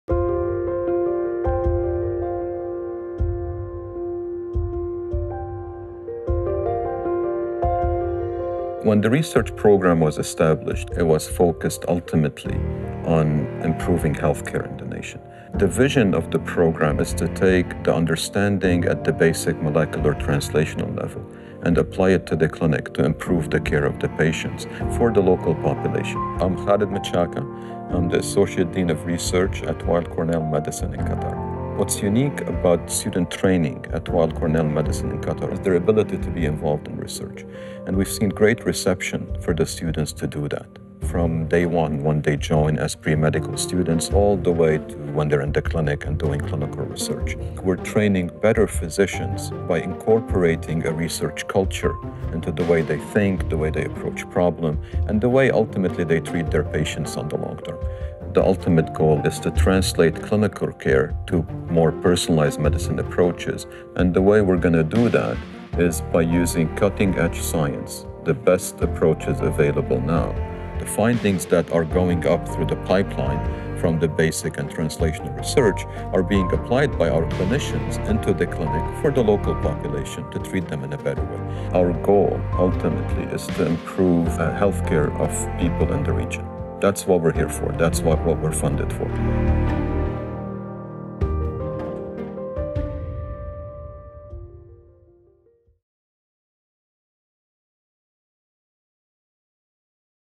Research
Welcome to the Research Division at Weill Cornell Medicine - Qatar (WCM-Q). We have established a state-of-the-art biomedical research program focused on tackling the most pressing health needs in Qatar and the region. We undertake biomedical research at the basic, translational and community levels, with the expectation that such a combinatorial approach will have a dramatic impact in addressing complex multifactorial diseases such as diabetes.

Excellence and professionalism are our most prominent themes, and we strive to adhere to them in all aspects of our operations, from the science we are producing, to our core facilities, administrative services, and training programs. The WCM-Q research program is already contributing to building both biomedical research and human capacity in Qatar, and will continue to do so in the future. We employ a multi-pronged approach focused on recruiting exceptional scientists, delivering robust training programs, ensuring advanced infrastructure within the core laboratories, and a collaborative approach to produce cutting-edge knowledge.